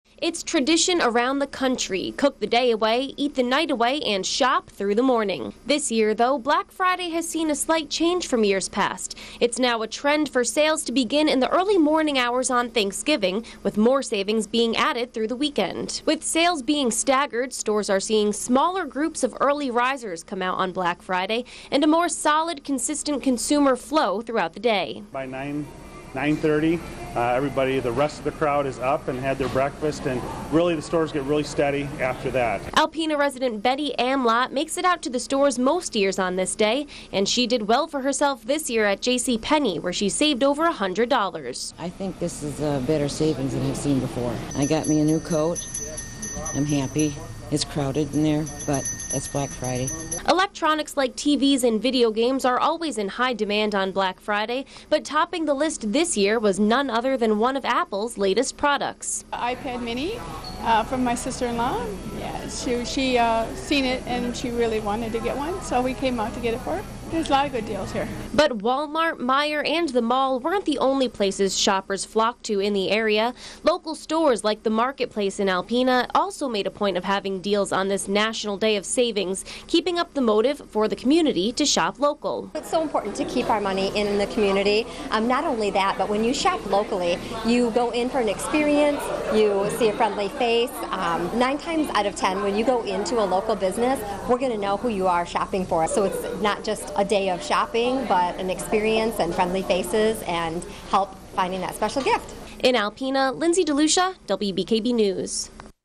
Steady Shoppers This Black Friday
It's tradition around the country: cook the day away, eat the night away, and shop through the morning.

This year, though, Black Friday has seen a slight change from years past – it's now a trend for sales to begin in the early morning hours on thanksgiving, with more savings being added through the weekend.